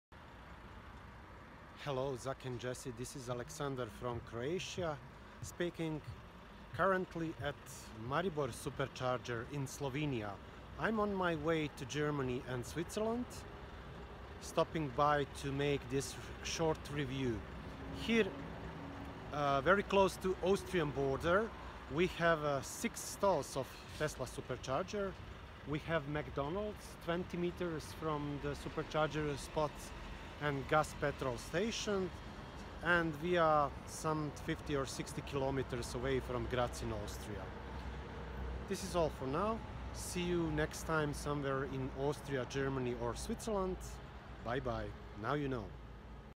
Tesla Supercharher in Maribor, Slovenia review
Short review of SuC located at northeastern part of Slovenia, close to southeast Austria border. McDonalds is just behind the charging spot and a Petrol gas station too. Easy access from freeway leading to Austria, from the south.